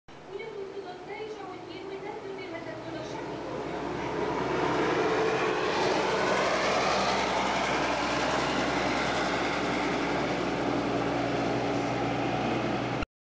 Volánbusz,Galyatető1999
Egy elsuhanó 256-os 1999 nyarán